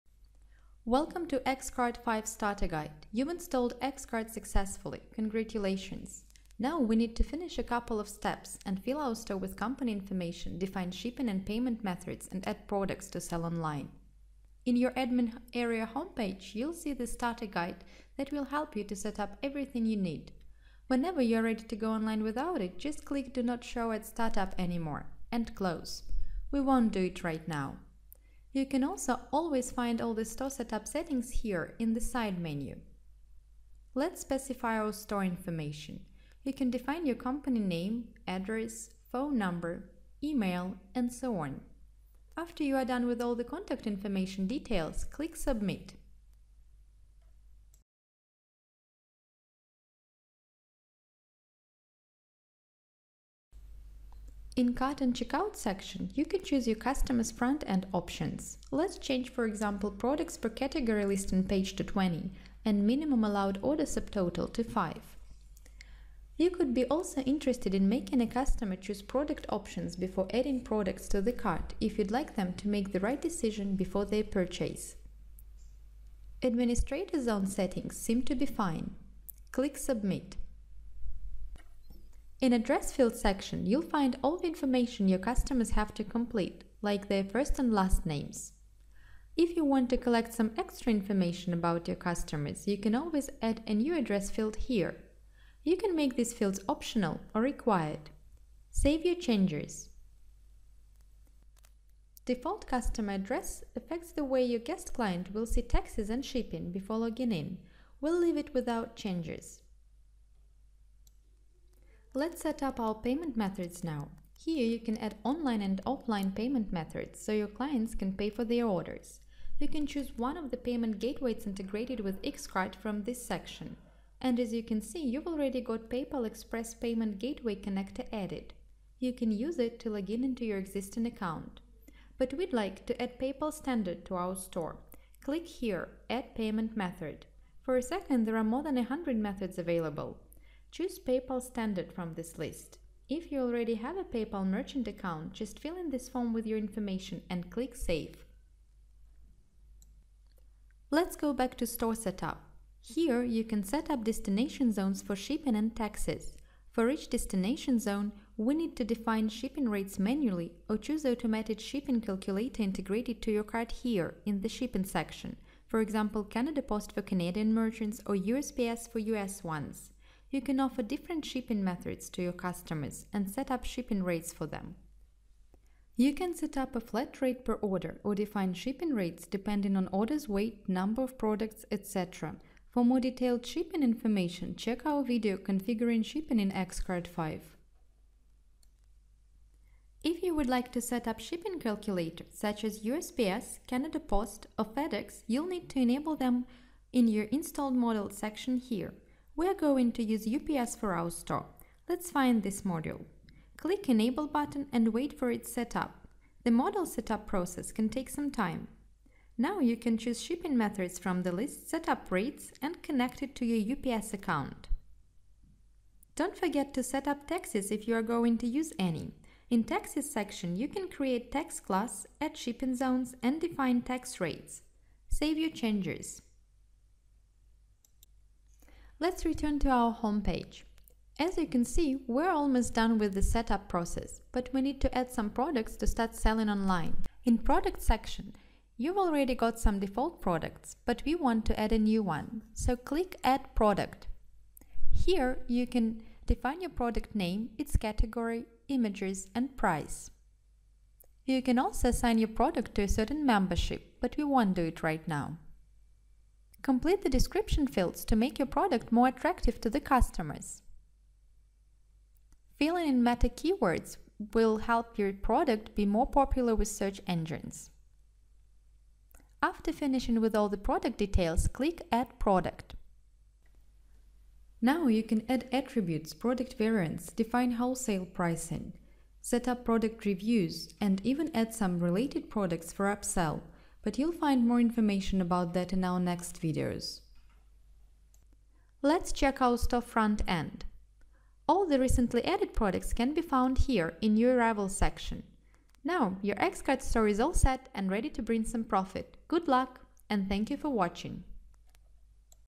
Getting started X-Cart v 5.2.x
After successful installation there are just a few steps left to complete to start selling online. This video will guide you through the process of setting your store up.
- your company info
- general store settings
- payment methods
- shipping methods
- adding products